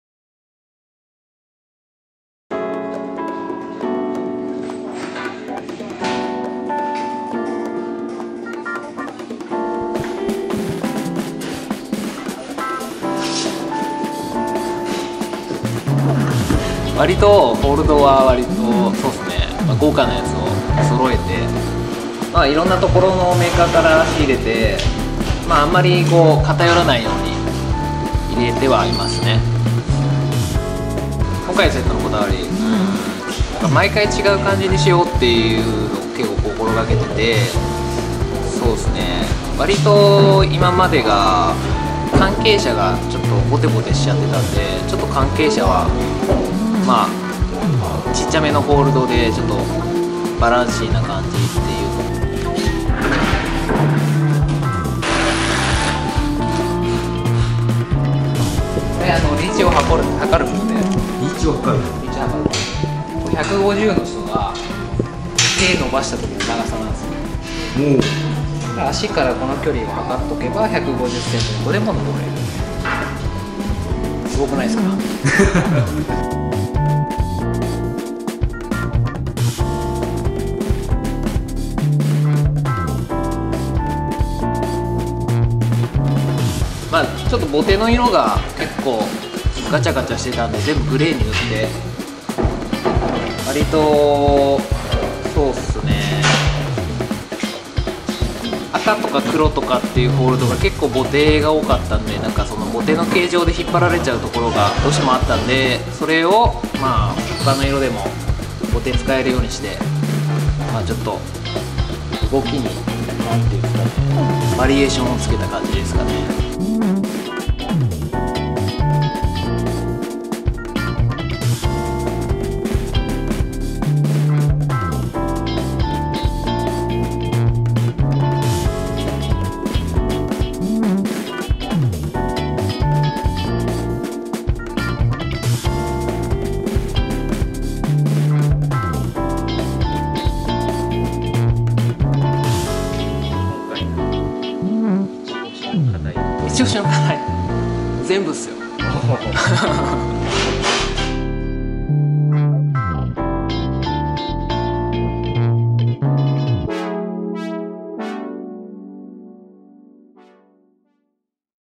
【ボルダリング】セットの裏側公開！D.Bouldering 八王子店のセットのこだわり【ホールドが豪華】
D.Bouldering 八王子のセット風景を密着撮影！

リーチ棒を使った女性が距離負けしない課題作り、ハリボテにラインを縛られないための工夫、Dボルダリング八王子店のこだわりとは？

八王子・高尾・北野でボルダリングならDボル！八王子駅北口3分！

クライミングジムでの撮影、動画制作のご相談はコチラから